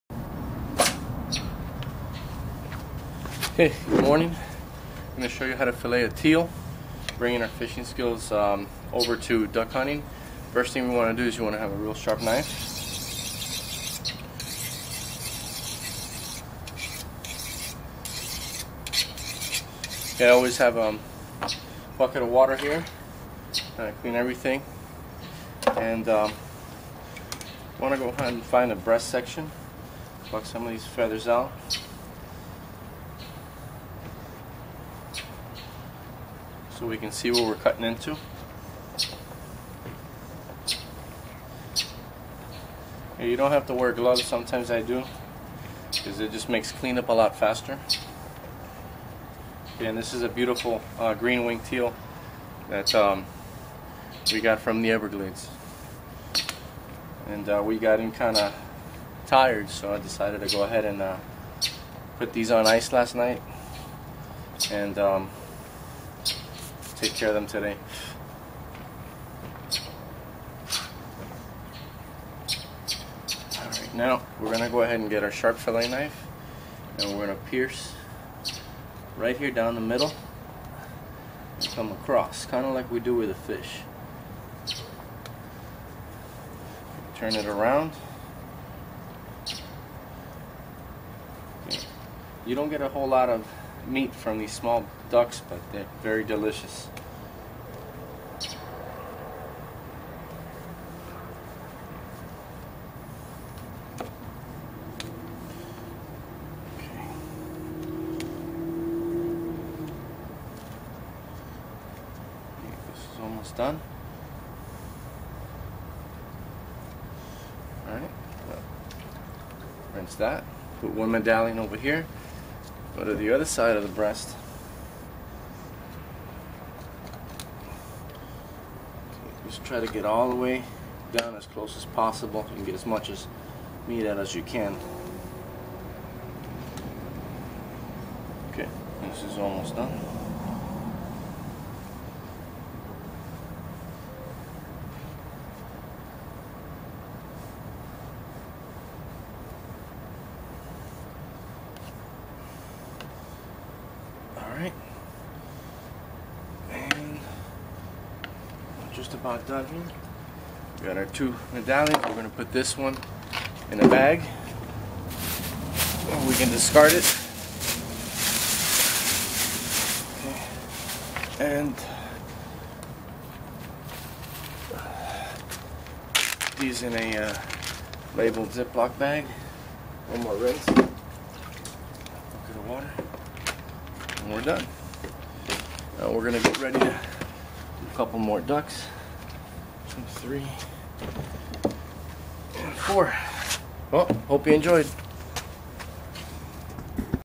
How to Fillet a Wild Duck
How to Fillet a duck using a fish fillet knife